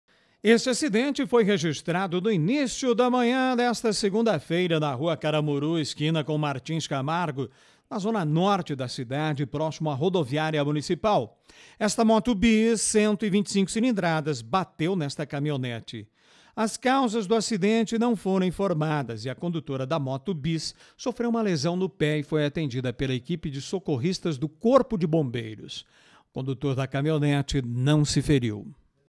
Registrado colisão entre moto e caminhonete
Segurança: Na manhã de hoje (09) foi registrado um acidente de trânsito entre uma moto e uma caminhonete.